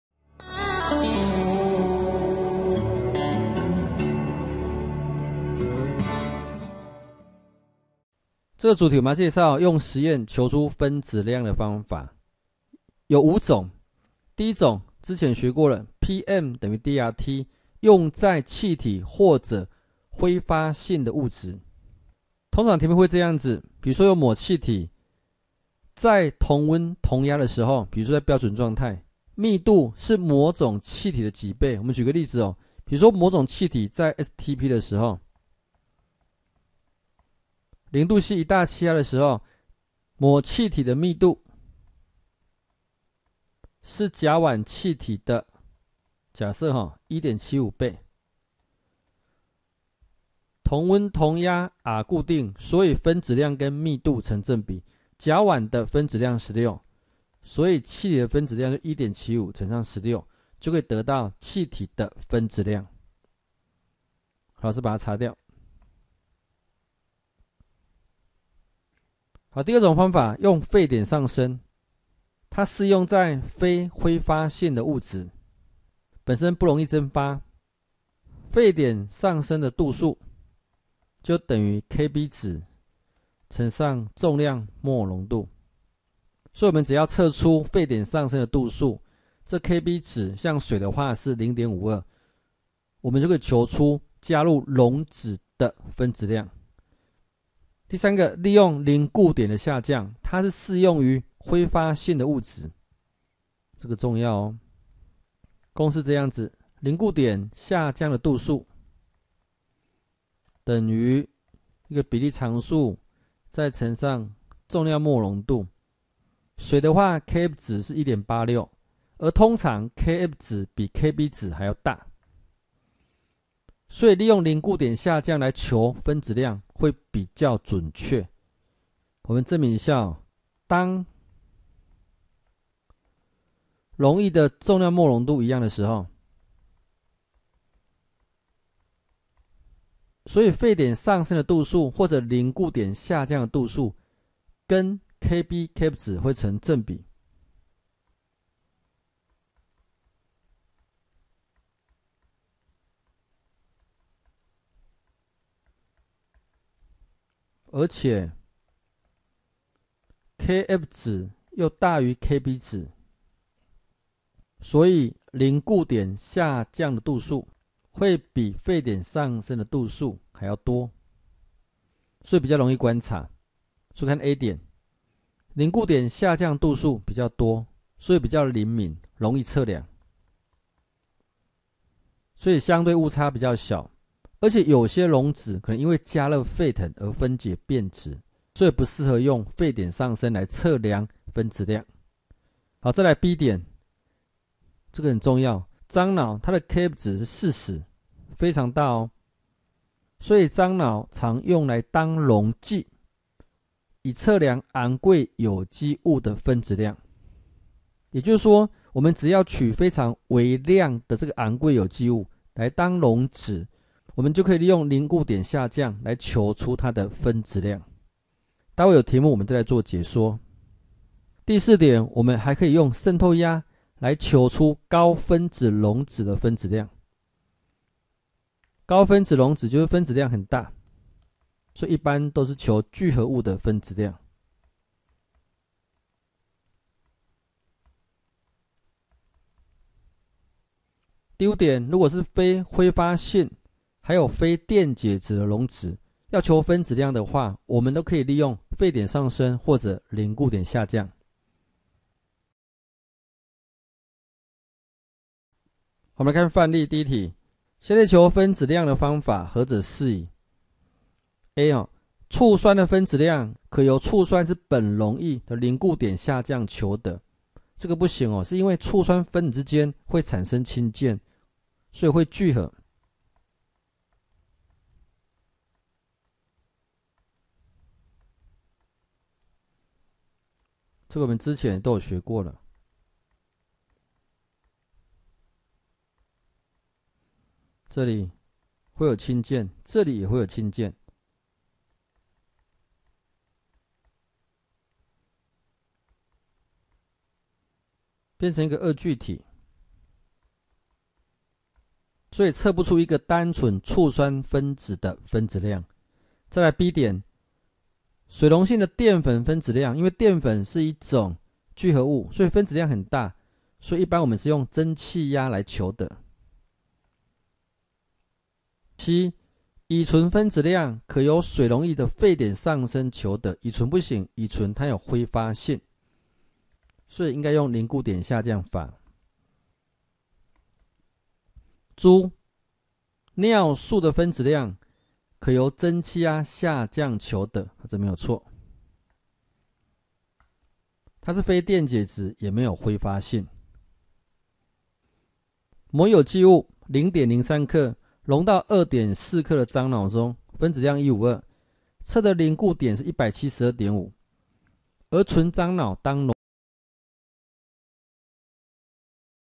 高中化學_選修化學(上)_液態與溶液:以實驗求分子量的方法【莫斯利高中自然科教學網】
1.PM=DRT:適用於氣體或揮發性物質。
2.沸點上升:適用於非揮發性物質。
3.凝固點下降:適用於揮發性物質，且Kf比Kb大所以易於觀察。
4.滲透壓:可求出高分子溶質的分子量。